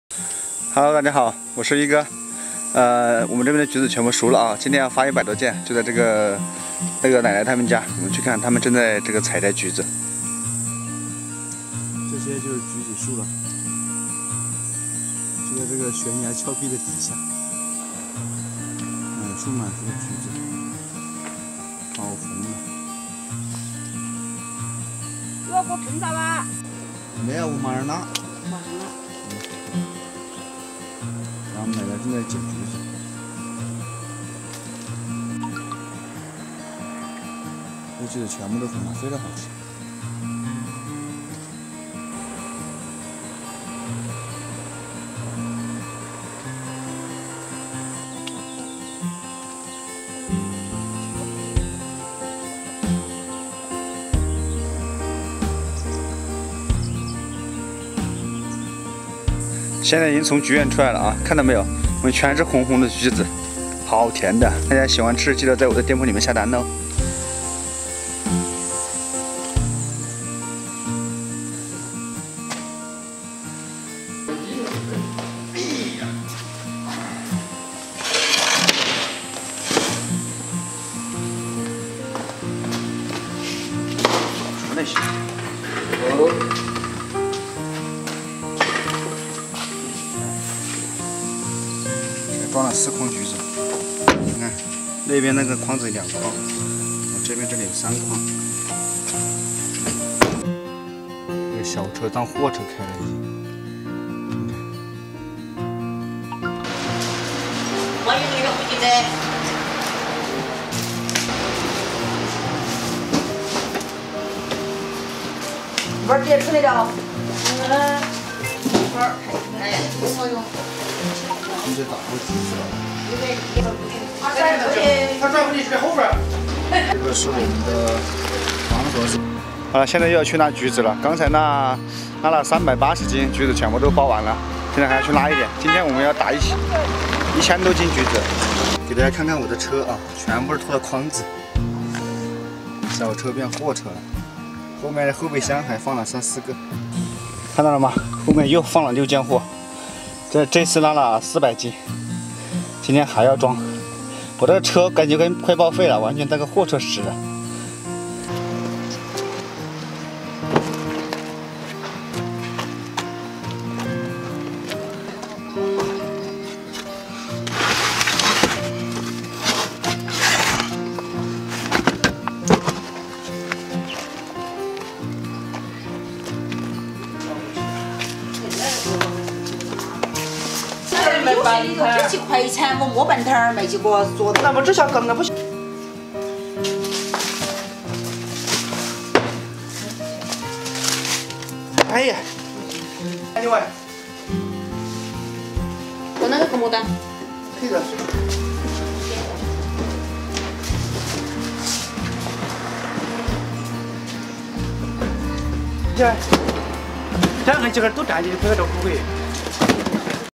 农村小伙帮助农户卖桔子，一天卖出一百多单，拉桔子轿车当货车使
小伙帮奶奶卖桔子，轿车当货车使，一车拉四百多斤，车都快报废